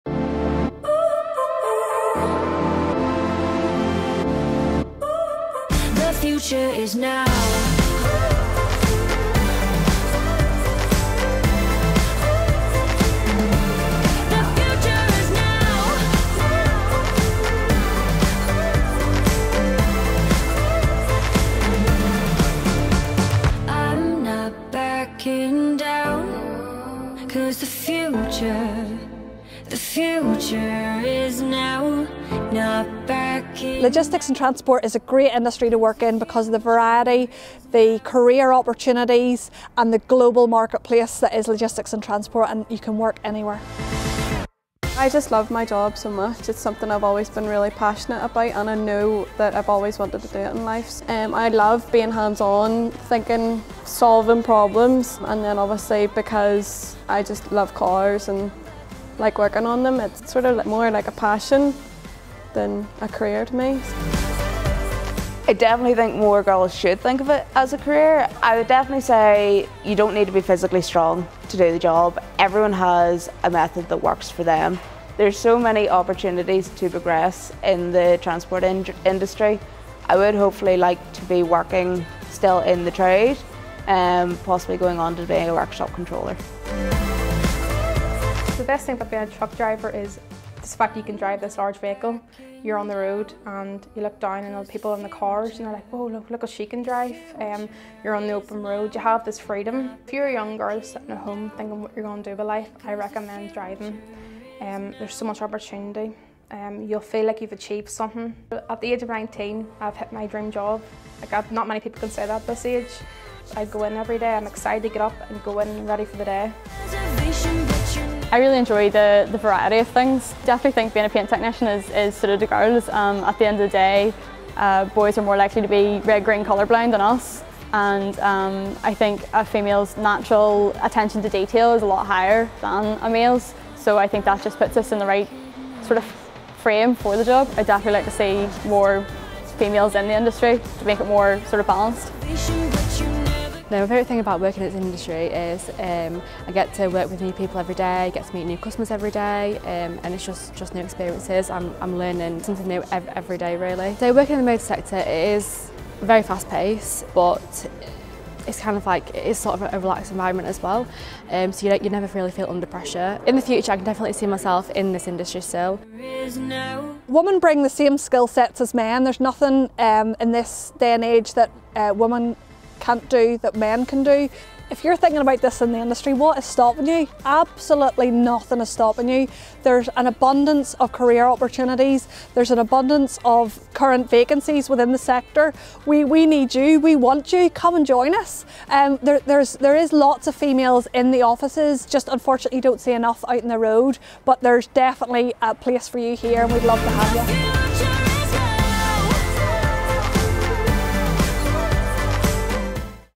Introducing Women in Wheels NI ✨
Females are highly underrepresented in the automotive, transport and logistics industries.  We want to change that!

Through Women in Wheels NI we are highlighting females that work in the transport, logistics and automotive sectors in the hope that we can break down some of the misconceptions about a woman’s role in these sectors and to inspire the next generation of young women to consider transport and automotive as a viable and exciting career option.

To find out more about these women driving change in the transport and automotive sectors in NI, check out the Women in Wheels NI Facebook group